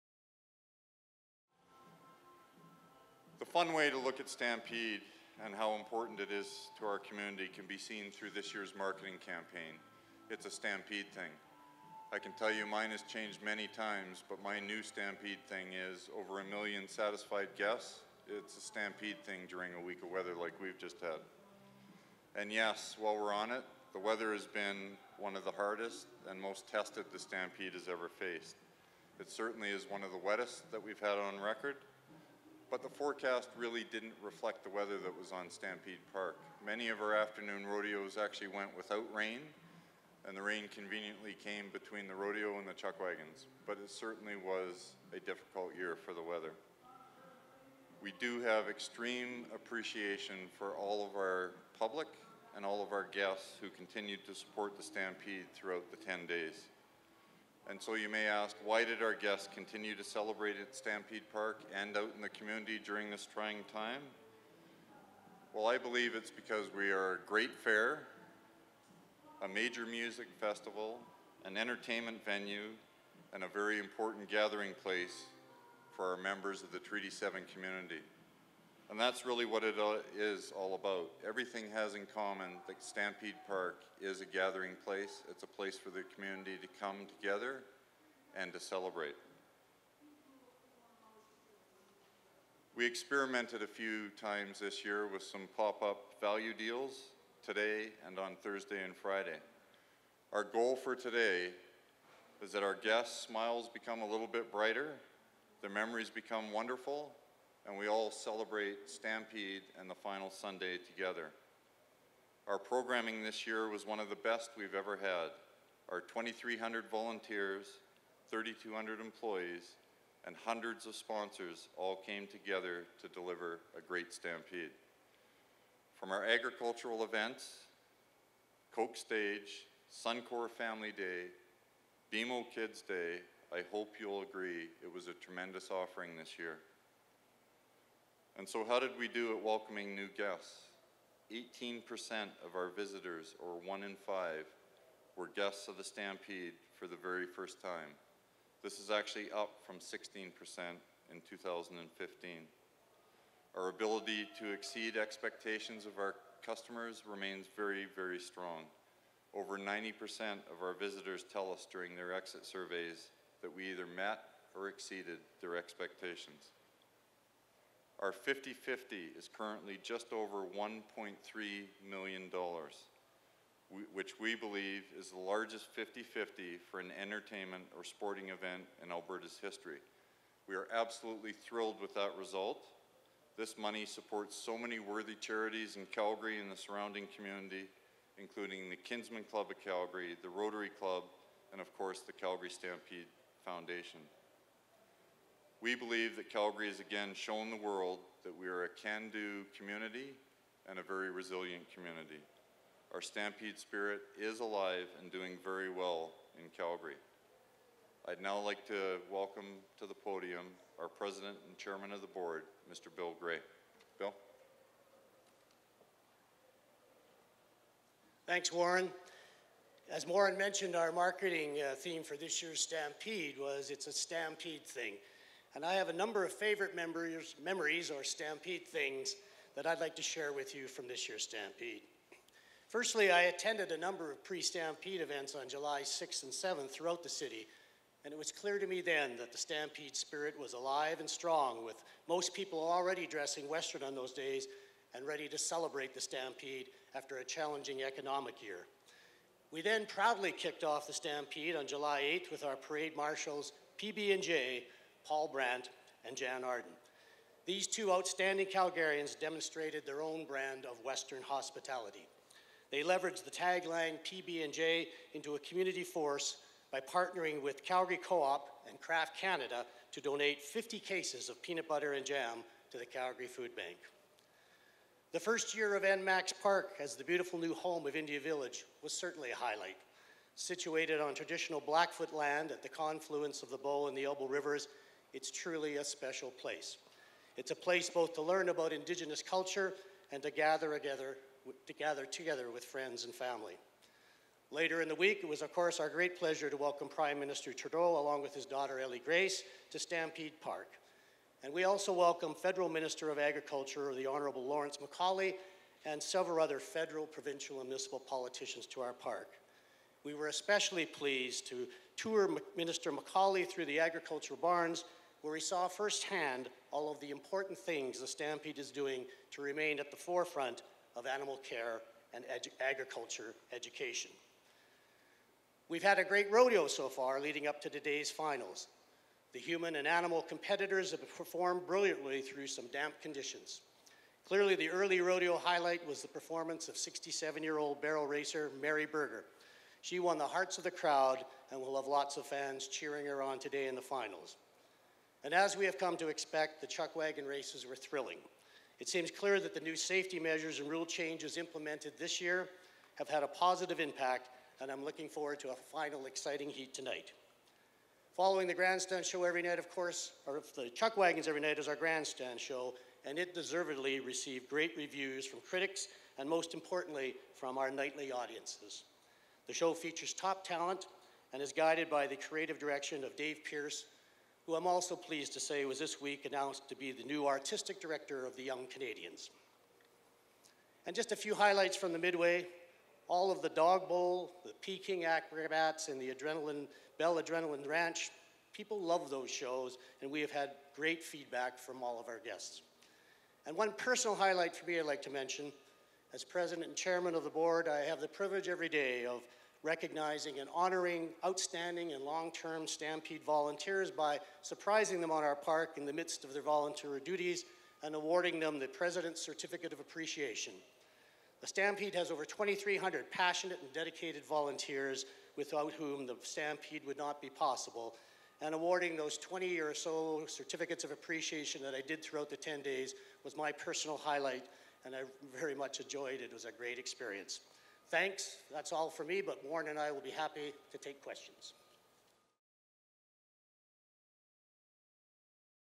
Closing remarks for the 2016 Calgary Stampede
President and Chairman of the Board, Bill Gray and CEO, Warren Connell share some of the highlights of the 2016 Calgary Stampede.